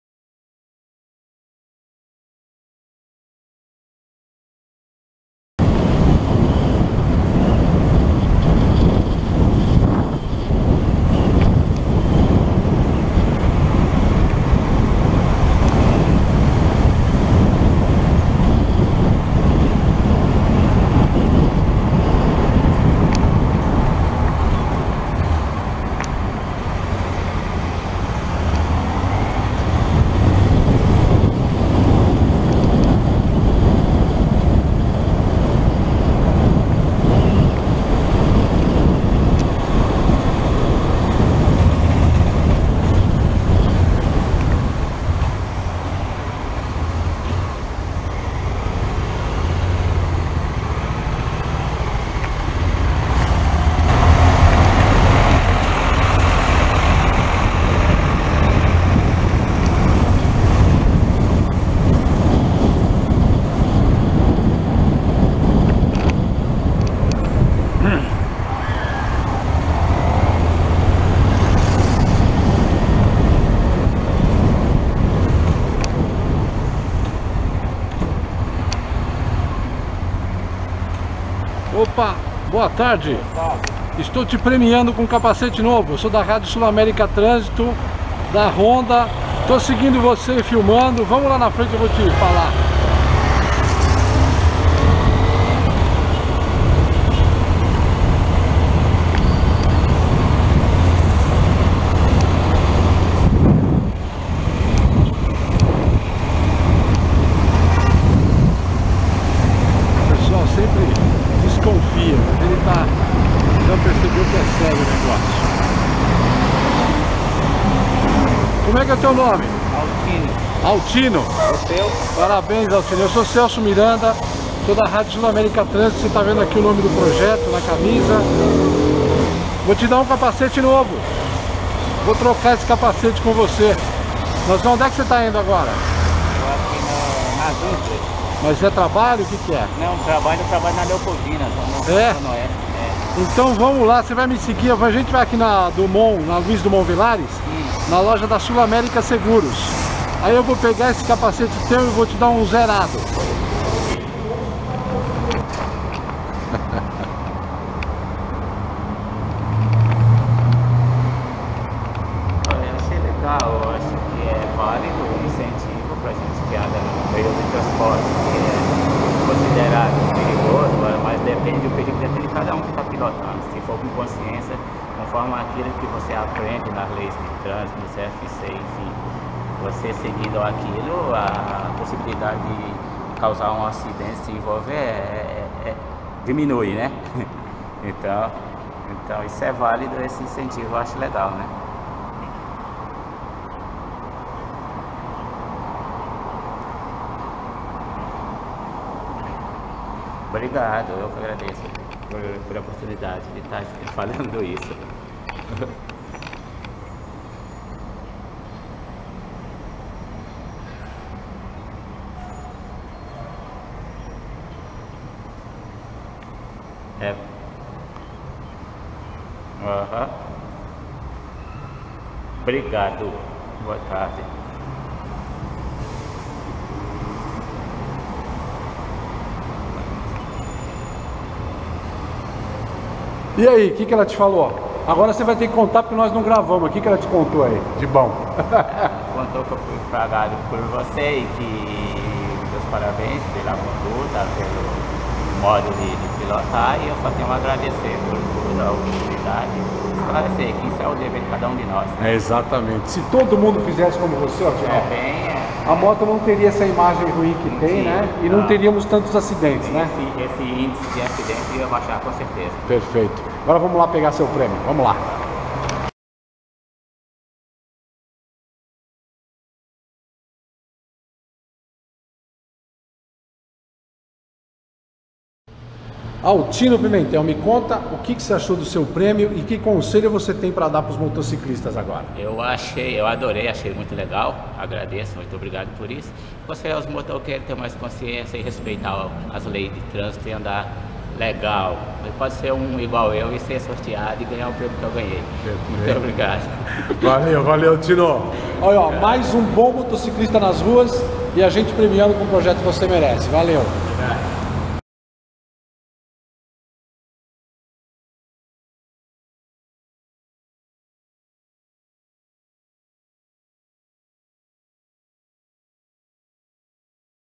PROJETO VOCÊ MERECE PREMIA ALTINO PIMENTEL 20042016
O Jornalista Celso Miranda saiu mais uma vez às ruas de São Paulo com uma ação do Projeto Você Merece criado por ele em 2011. Ele encontrou o motociclista Altino Pimentel nas ruas da zona norte da cidade. O Você Merece visa dar reforço positivo para os motociclistas que usam a motocicleta como meio de transporte sem abusos e respeitando as regras do trânsito.